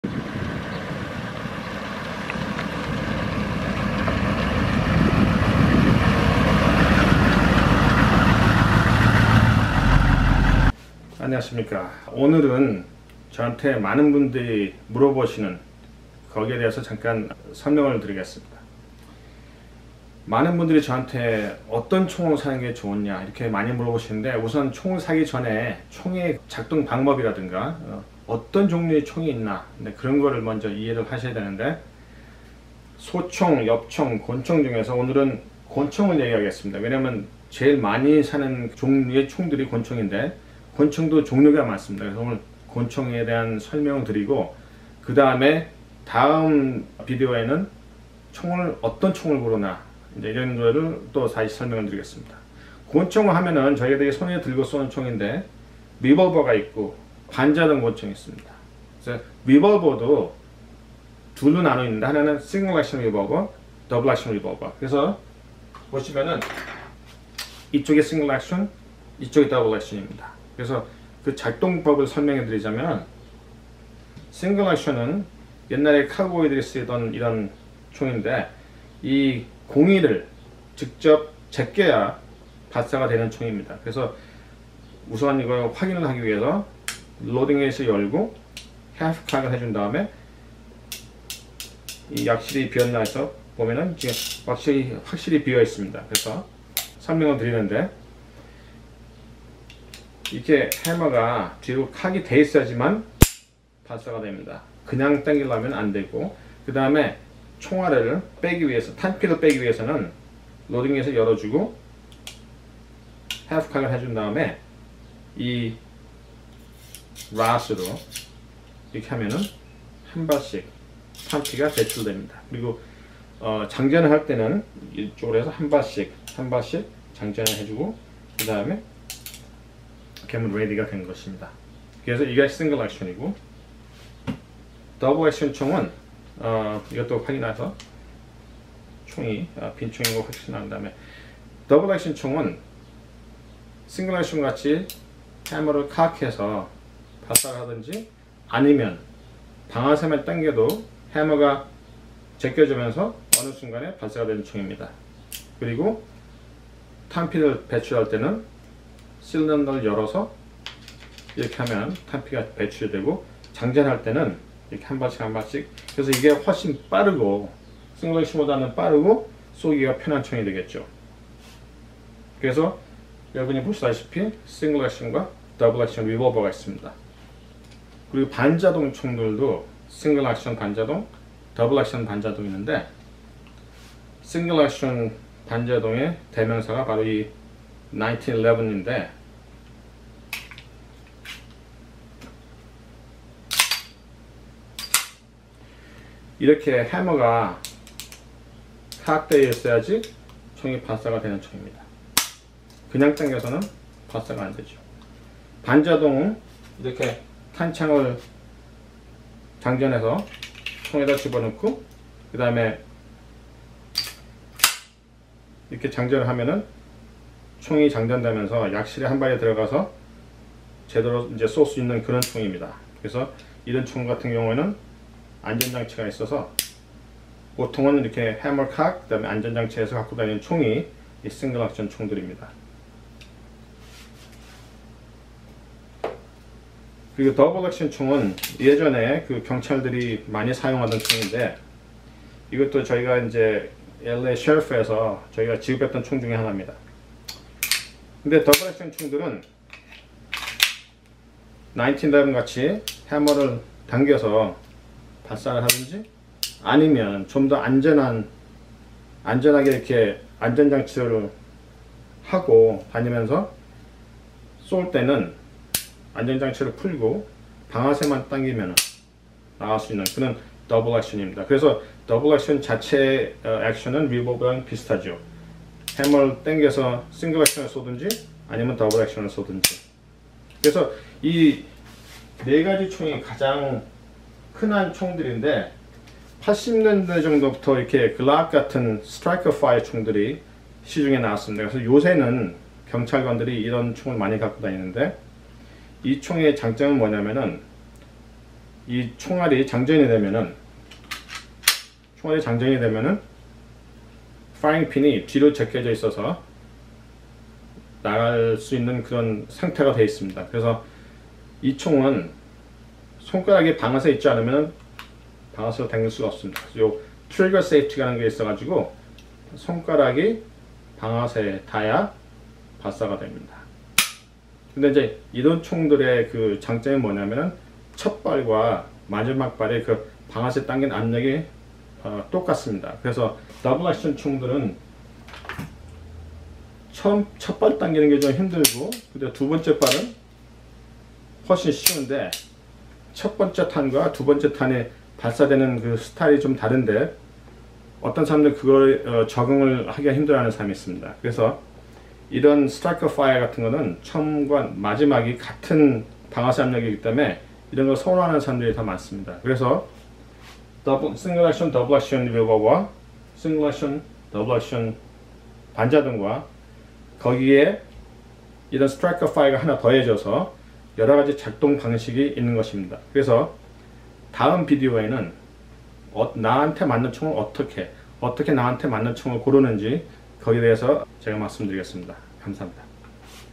Understanding Handgun Types/ 권총의 종류에 대한 이해
총기를 구하기 전에 권총의 종류에 대한 이해가 필요함. 리볼버와 반자동 총기